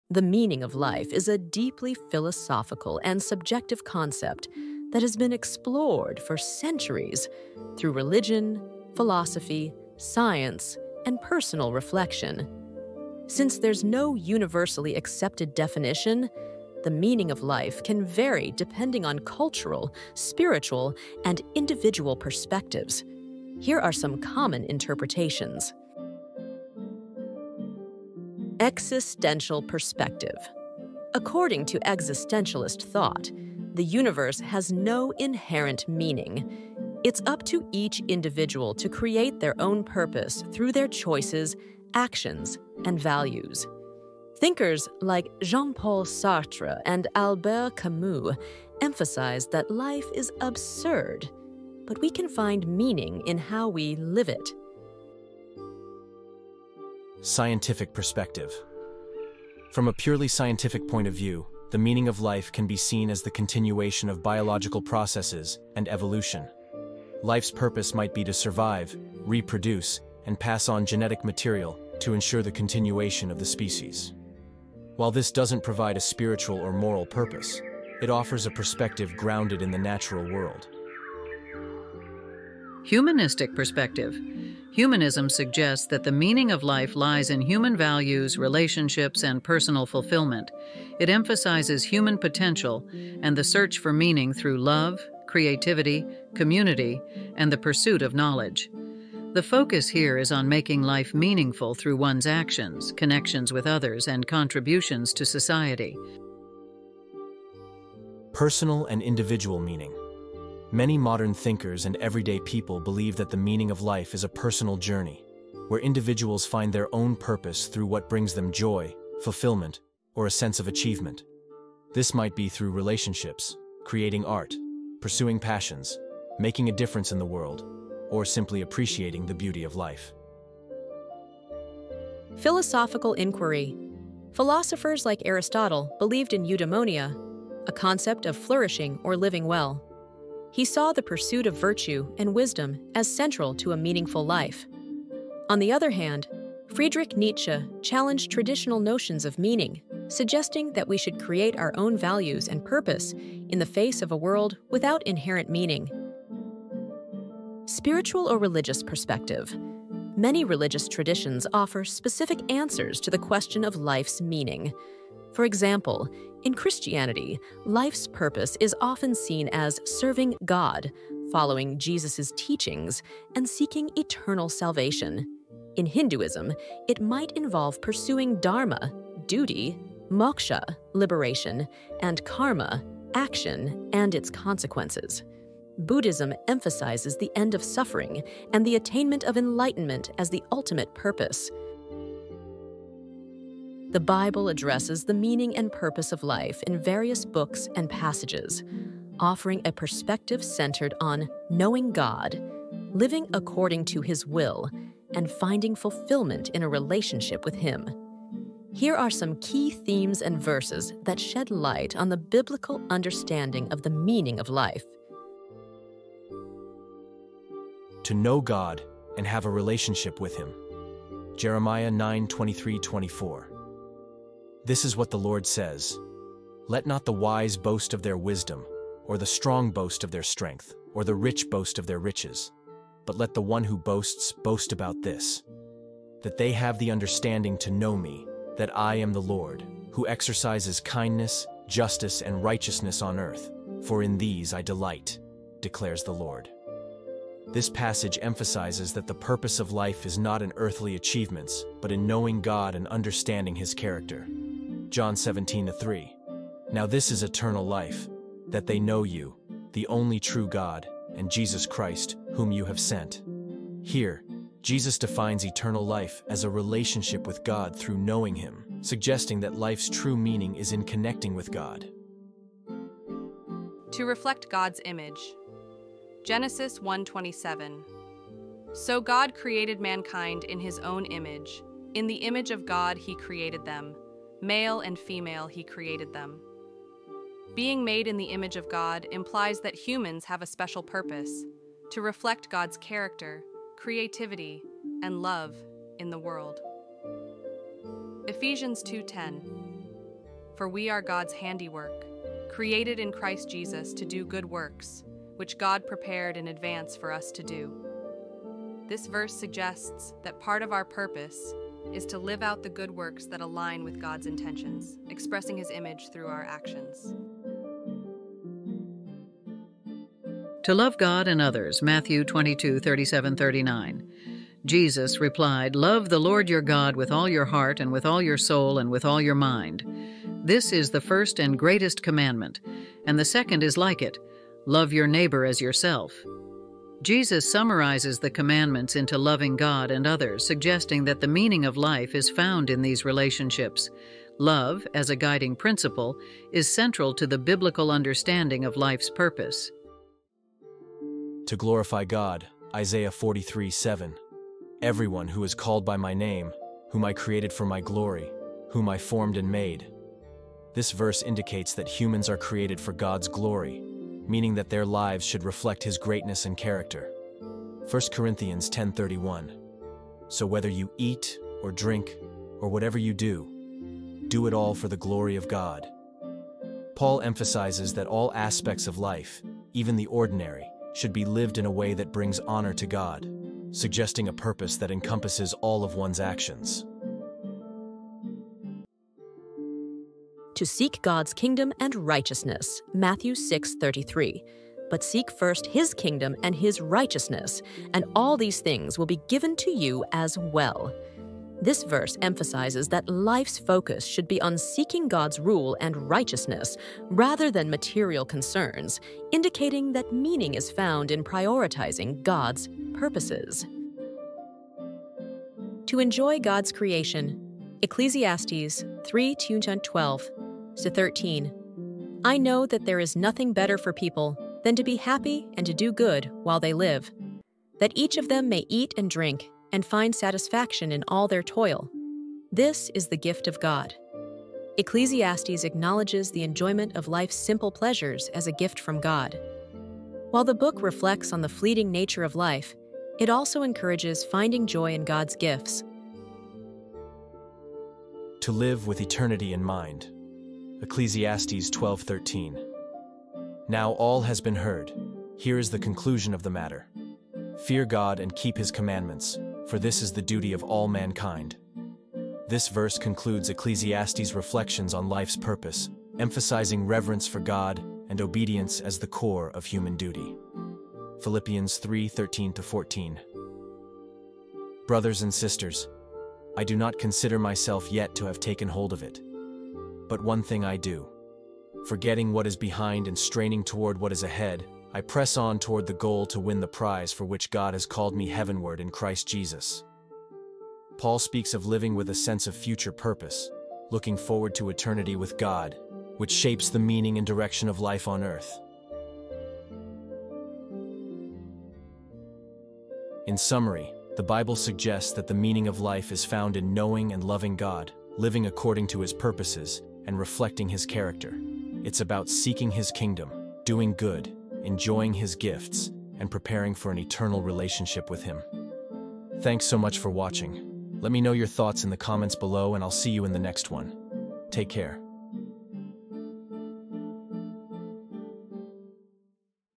Knowing your purpose and why you were created
The meaning of life is a philosophical question. Some say it is personal and transitory, while others believe it has a permanent objective meaning.
In this video are some ideas about the meaning of life.